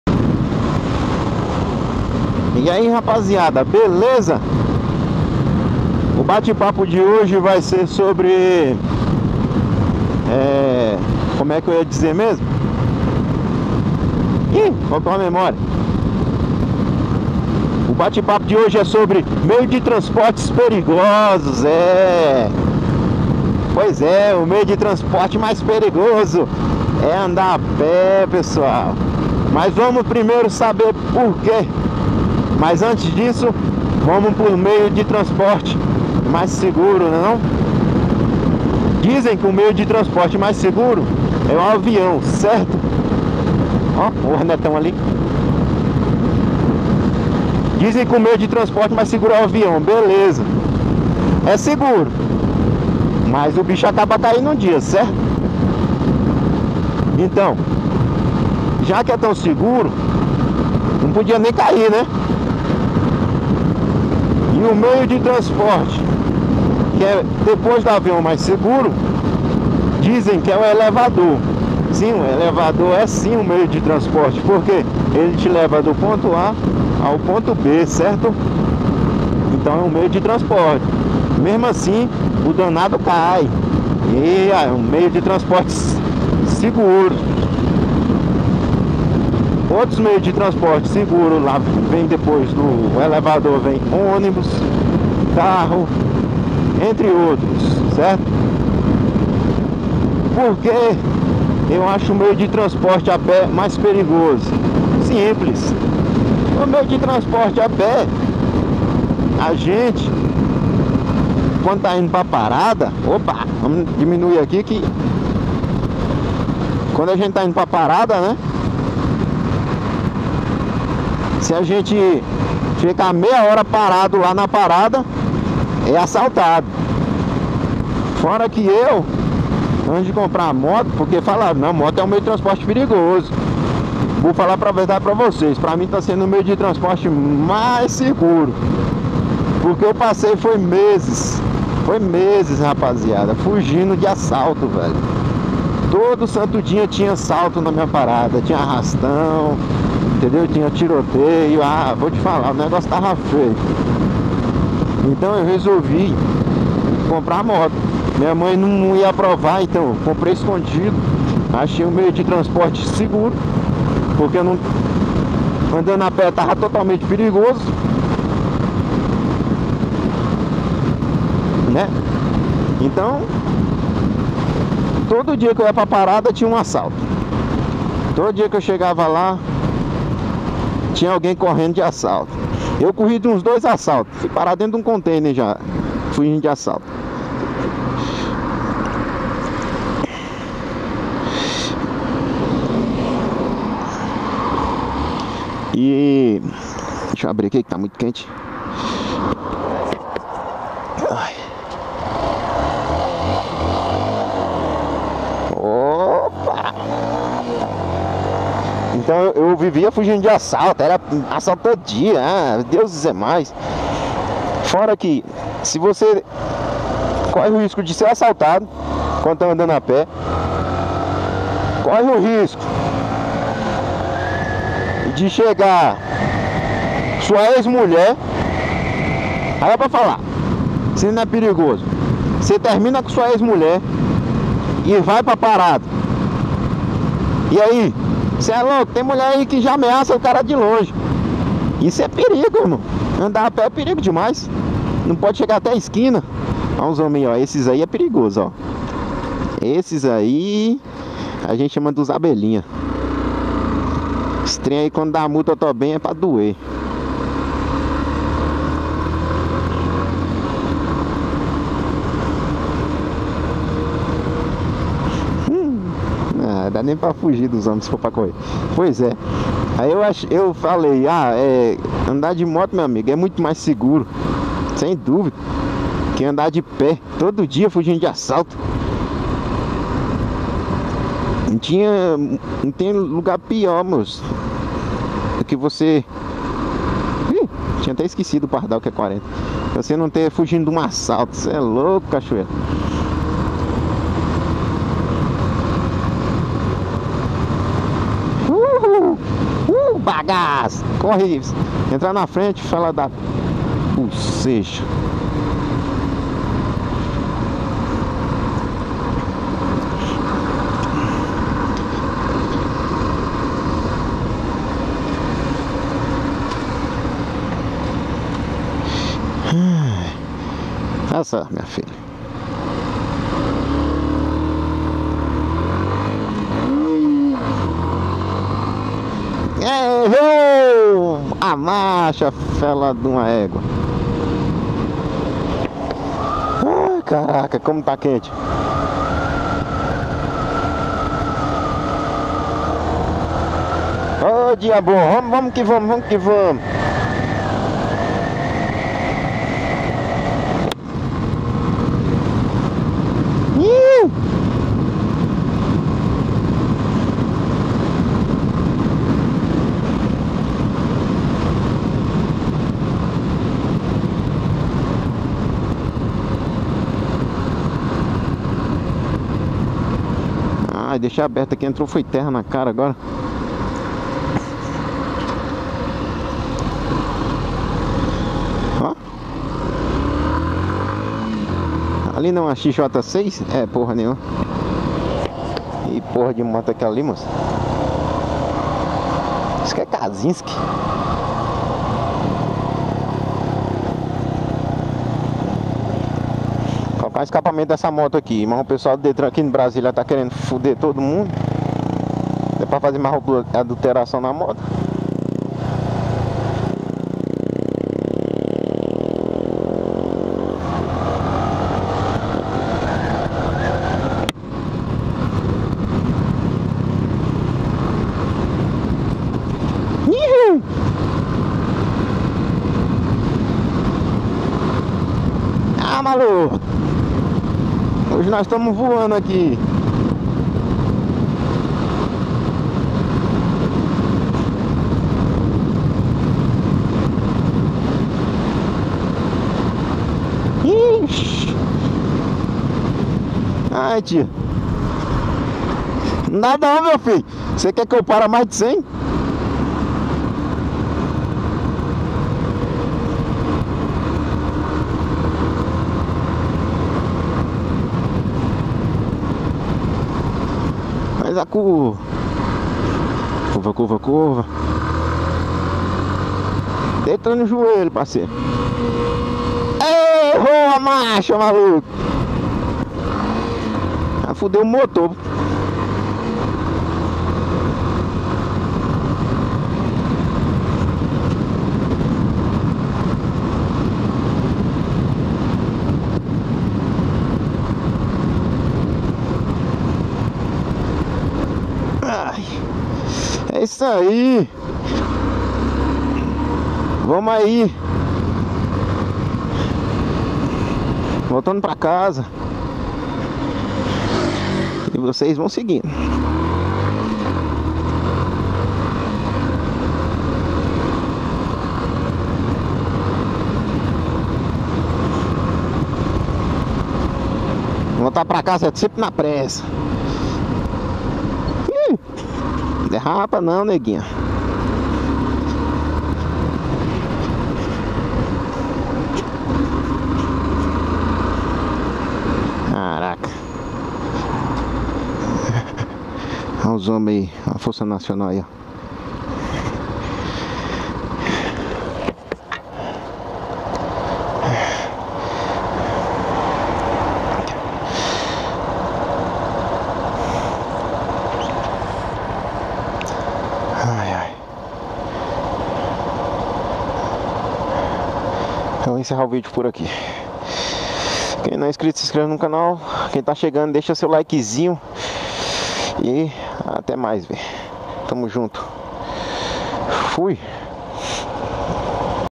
SERÁ QUE MOTO E MESMO PERIGOSO ???
Esta e minha opinião sobre transporte e segurança no nosso país .

Comentem da aquela força pro canal crecer tmj .

Moto: filmado de Brasília Distrito Federal.

Moto:.Factor 150 /2017.

Cor .:vermelha com preto.

Filmagens :Rotinas no dia a dia.